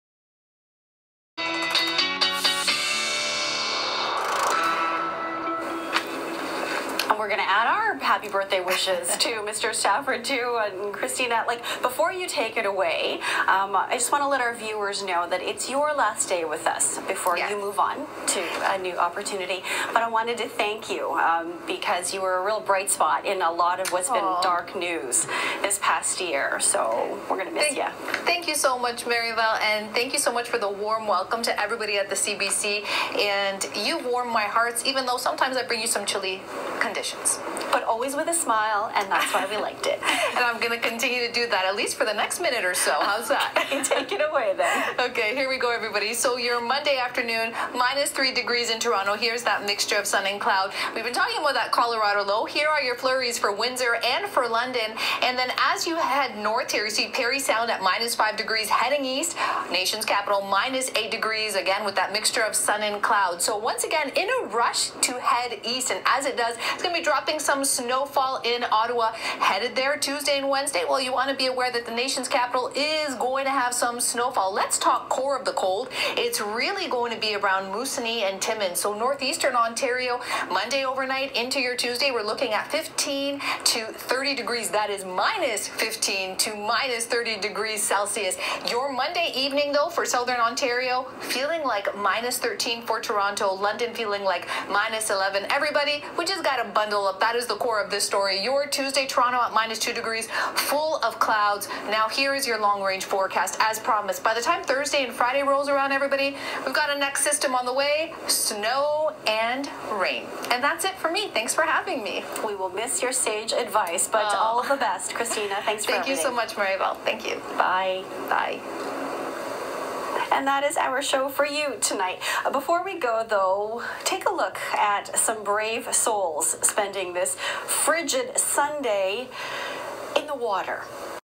My last Weather Update 🇨🇦for the CBC/Career Highlight!
My last weather ⛄ update for the📺  CBC 🇨🇦 comes with a life lesson... plz see vid

This is the story of a smile. 😊 plz see vid

One candle 🕯️can light endless more without burning out- and a smile can reach millions without fading. 💖

Thank you for having me - Ill be seeing ya 🤗